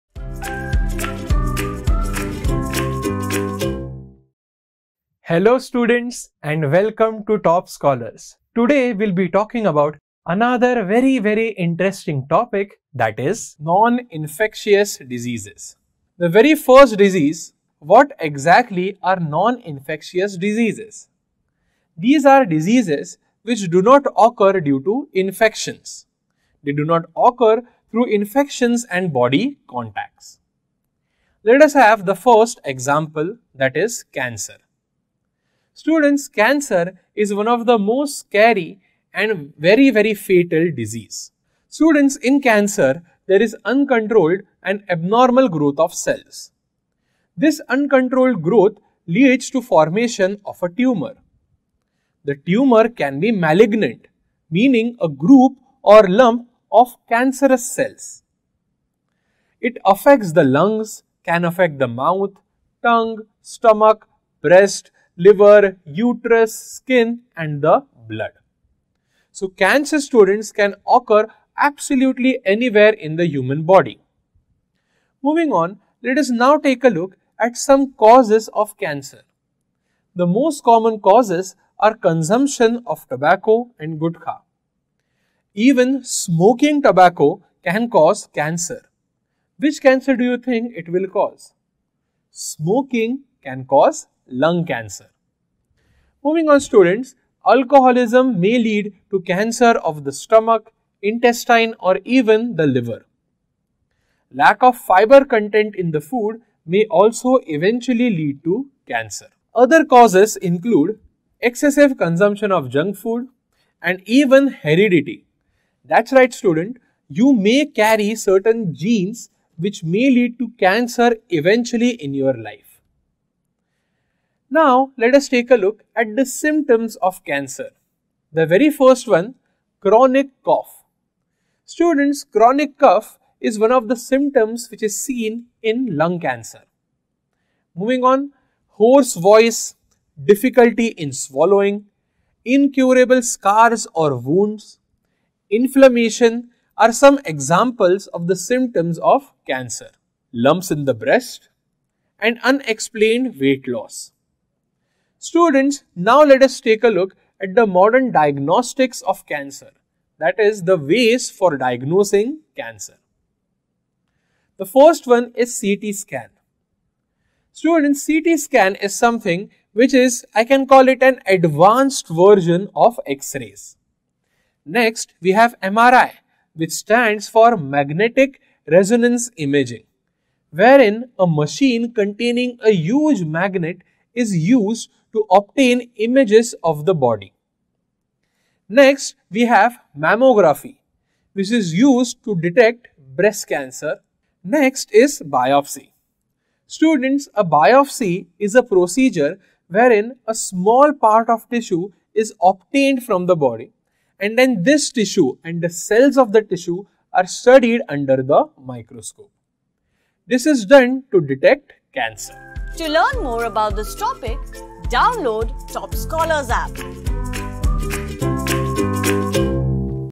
Non-infectious diseases | Biology | Disease | Disorder | Syndrome
Non-infectious diseases do not occur through infections and body contacts. non-infectious, infectious, diseases, disease, deficiency, genetic, disorder, syndrome